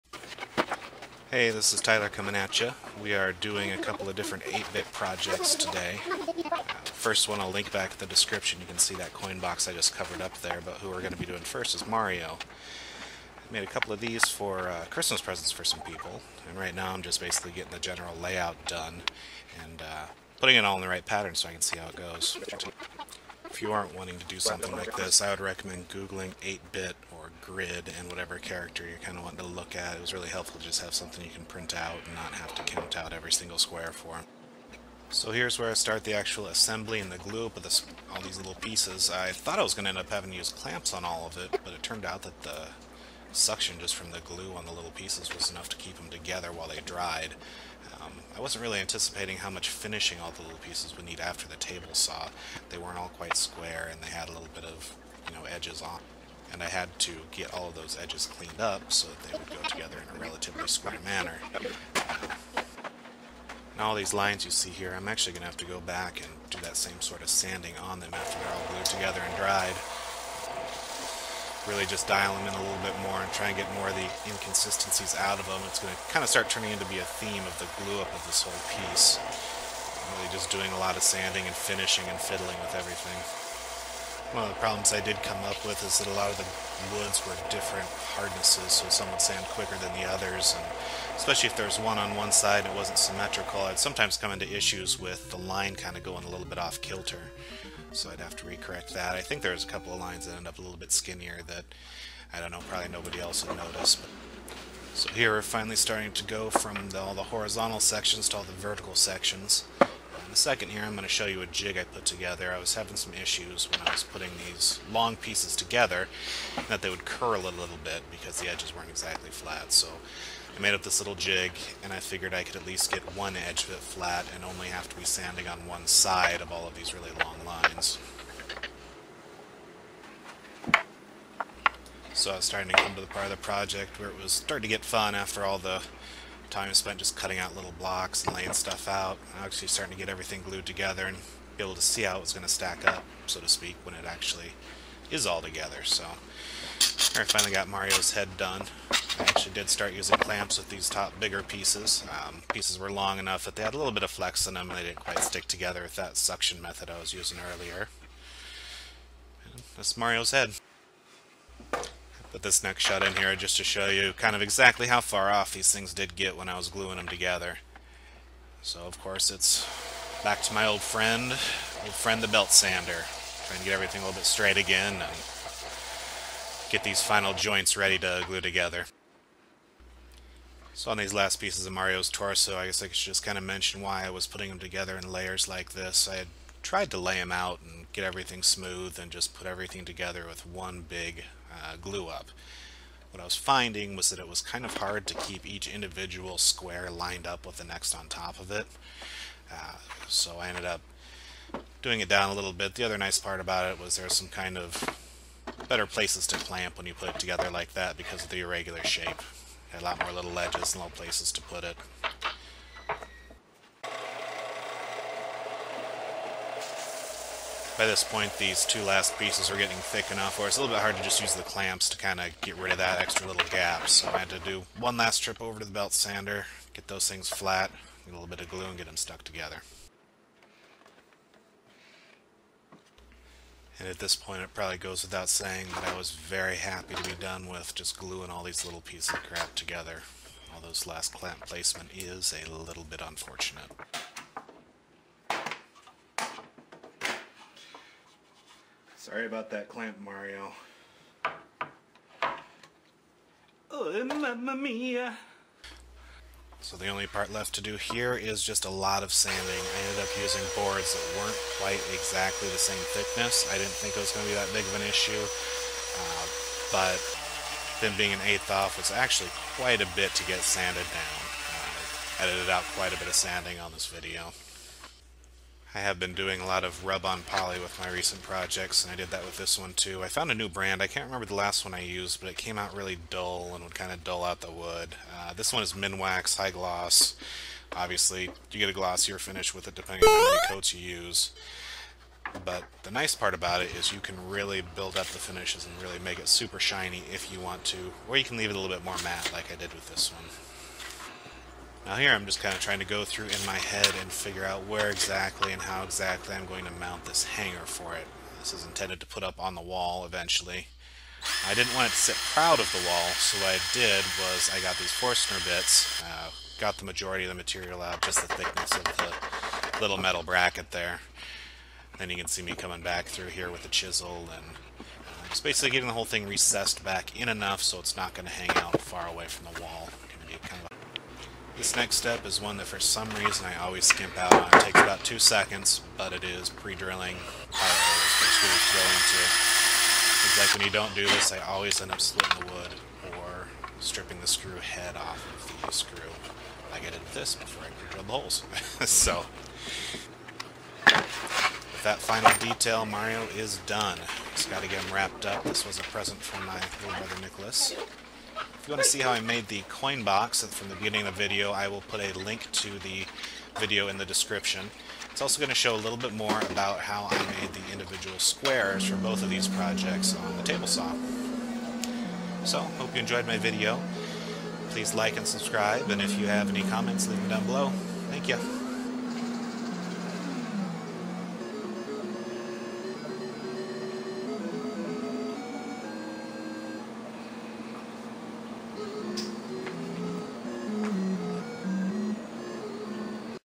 8-bit Mario in Wood part 2/2
I'm trying something a bit different in this video and doing a voiceover. I have gotten a lot of feedback from people that thought it would be a good addition so  we are trying it out, would love to hear which way you prefer.

Check the first part of this video out for some insight on how I cut out the blocks for this project and the different technique I used to put together the coin box.

In this second part of the two part series I get to gluing mario together. Both of these videos are about x-mas present I put together for this last year. Most of the description from the previous video applies here, I used the same wood (aromatic cedar, maple, and walnut) to get the different colors, which were actually a bit better suited to Mario's pallet then the coinbox imho. glue up was obviously a different technique which definitely worked better for the more irregularly shaped mario where I didn't have a border to butt it up against, and it also let me sand the end pieces while they were still in individual lines (with power tools)